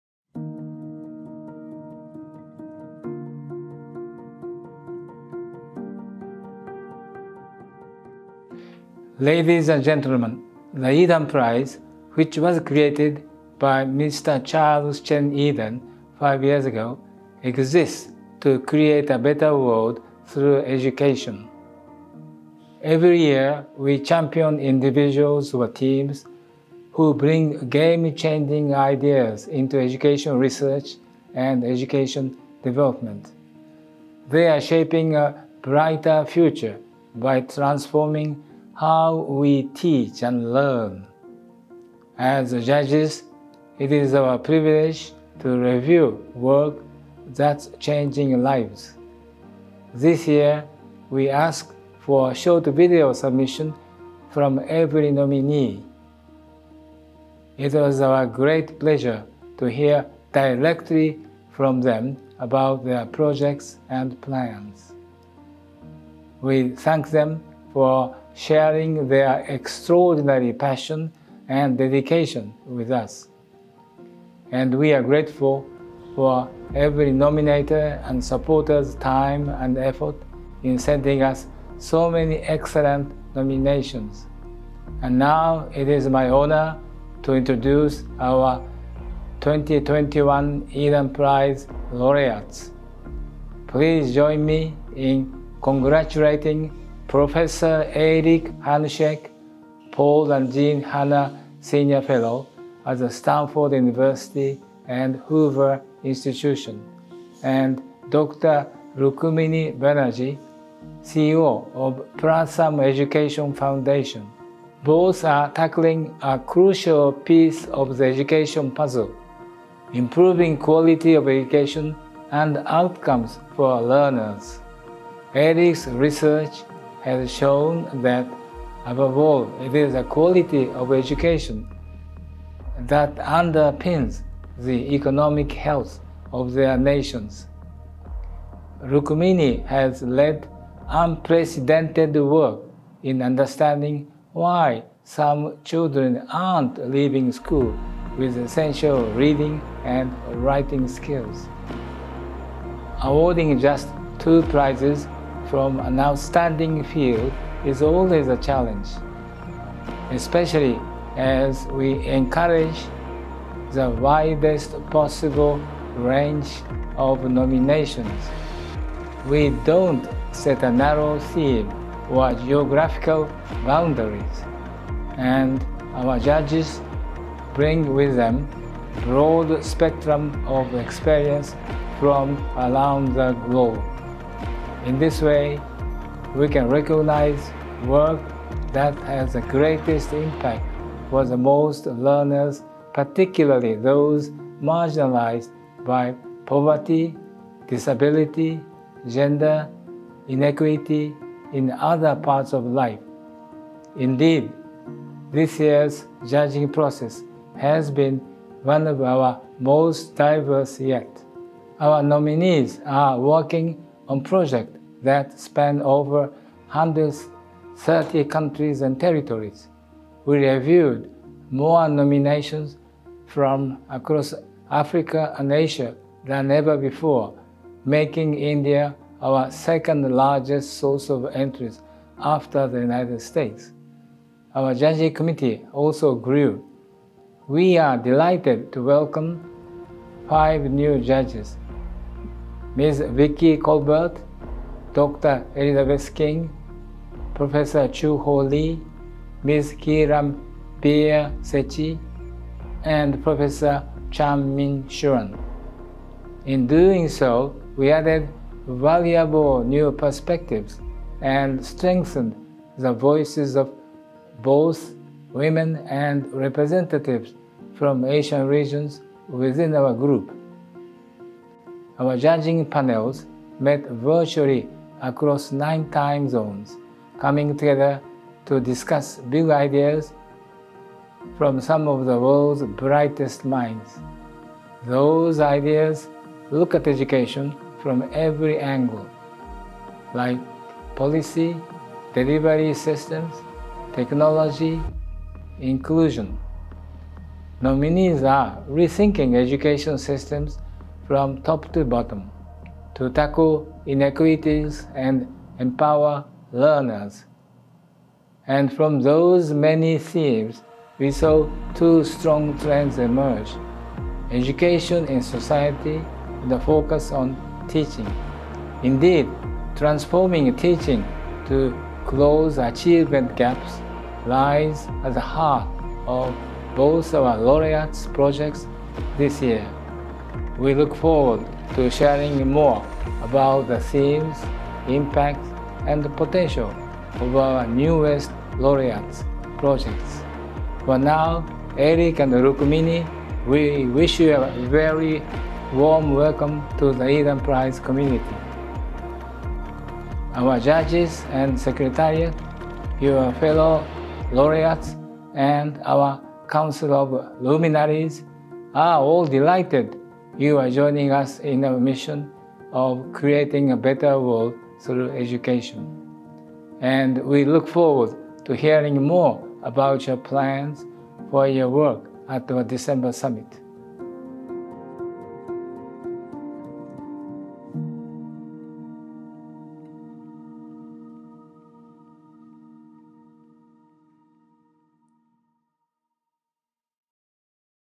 Welcoming our 2021 Yidan Prize Laureates | Dr Koichiro Matsuura
After six months of careful deliberation by our independent Judging Committee, we’re delighted to introduce to you our 2021 Yidan Prize Laureates.

Professor Eric A. Hanushek – 2021 Yidan Prize for Education Research Laureate
Dr Rukmini Banerji – 2021 Yidan Prize for Education Development Laureate